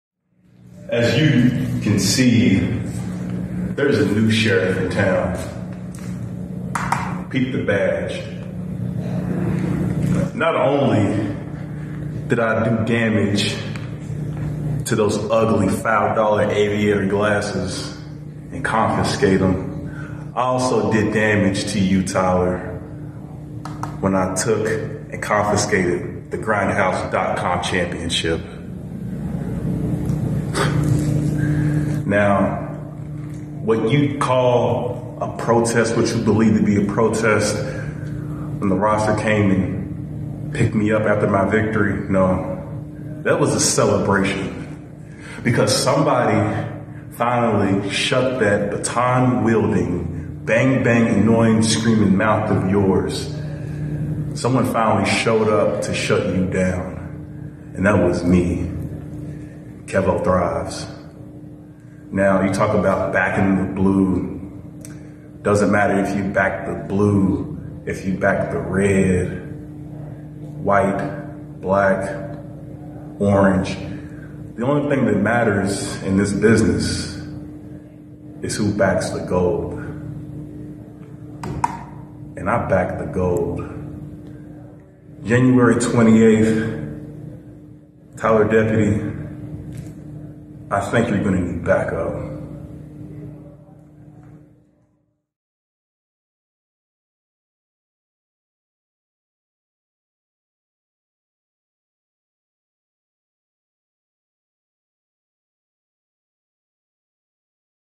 Back The Gold
Kevo Thrives has a renewed confidence heading into Rumble Rumble and his rematch with Tyler Lee Deputy THIS SUNDAY

Sunday, January 28th at 3 PM
The Arena
1416 Spring St
Jeffersonville, IN